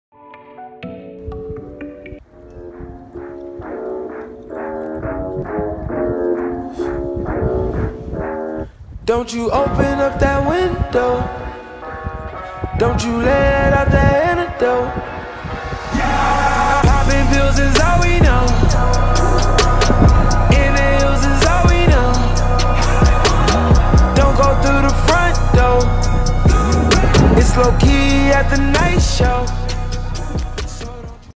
Сьемки перешитых сидений
Приехали сиденья , маленький тизер съёмочного процесса, ждем видосик.